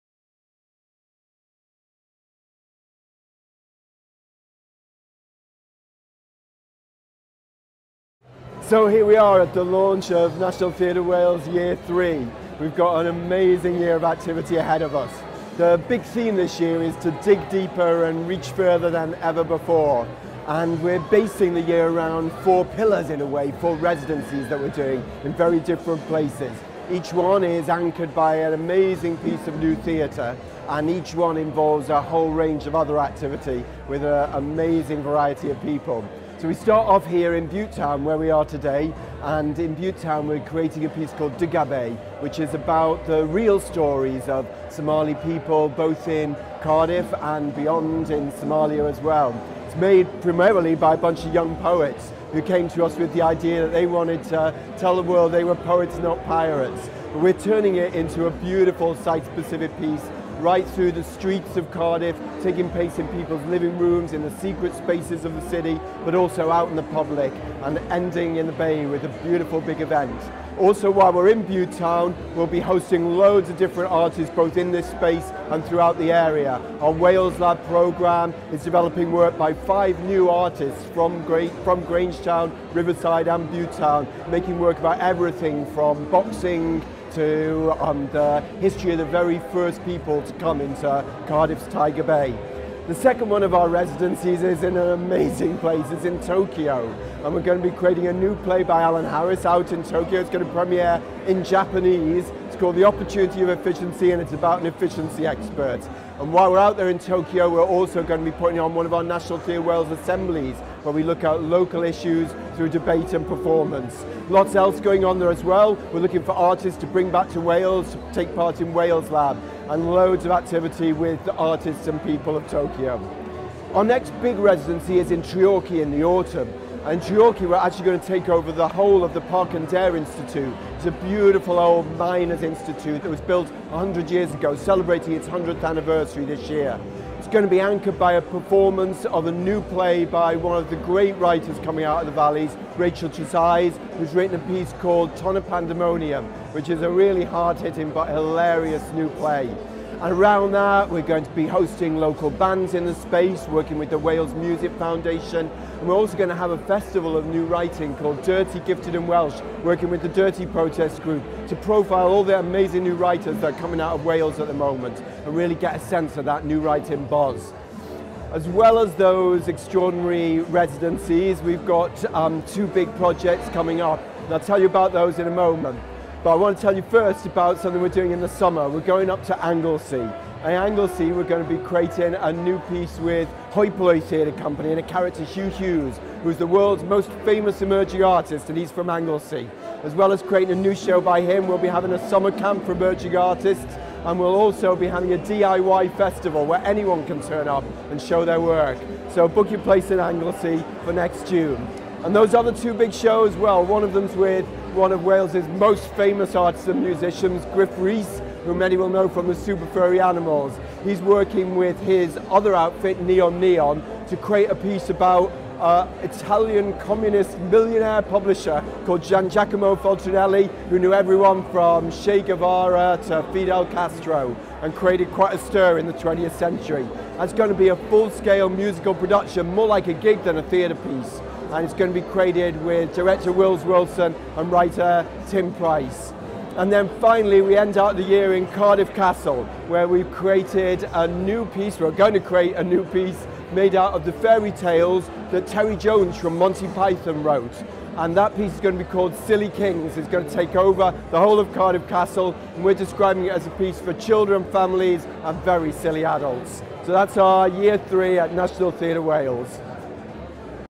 John McGrath introduces NTWY3
John McGrath, National Theatre Wales' Artisic Director, talks about what you can expect from NTW in 2013.